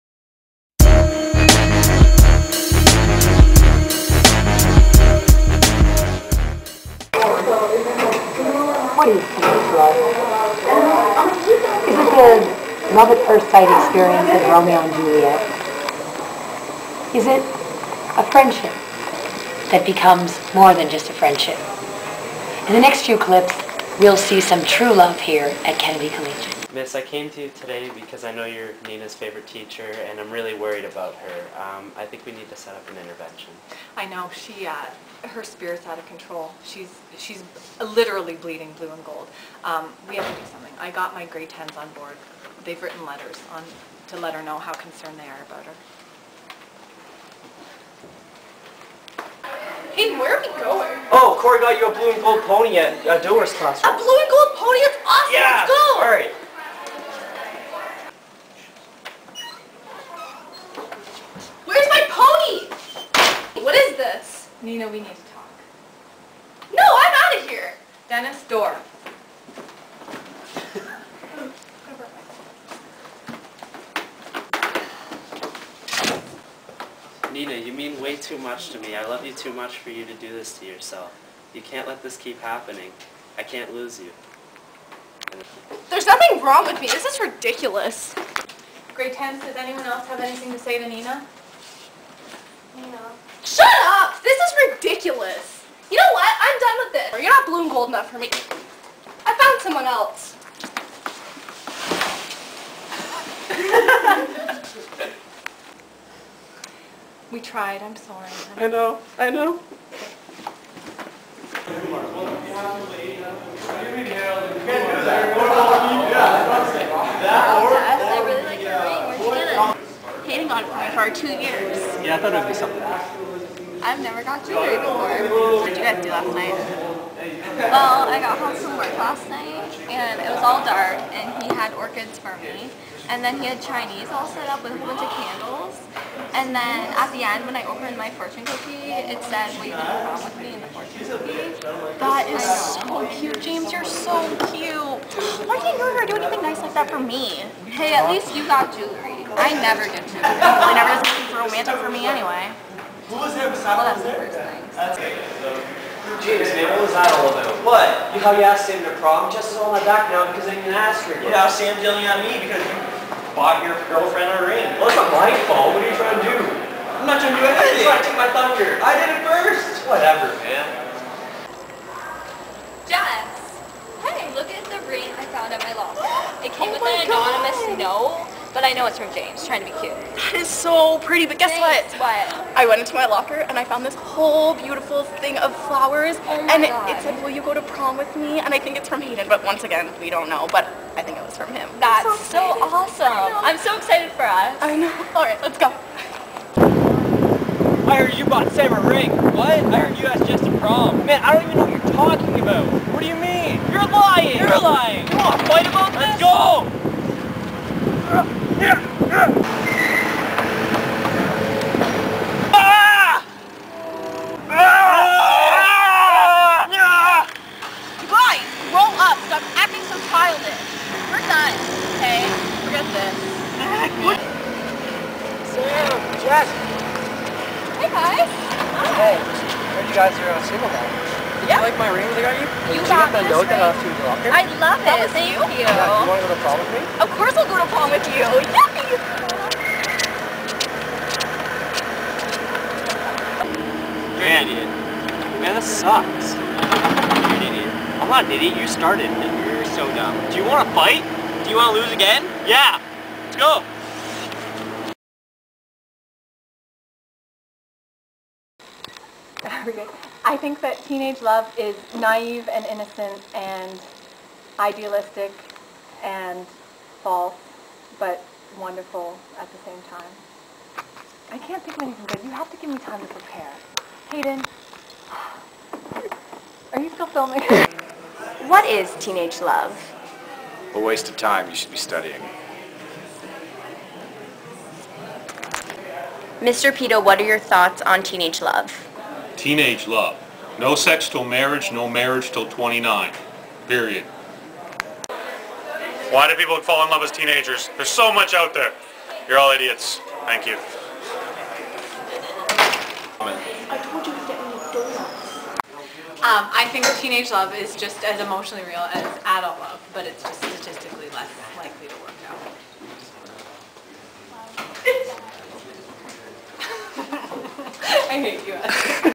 SCA 2011 CUTEST COUPLE
NOMINEES: NINA MILIDRAG & CORY SIMARD, JESSICA WORLEY & HAYDEN FREKER, SAM CAMPEAU & JAMES BEDNARIK
WINNER:NINA MILIDRAG & CORY SIMARD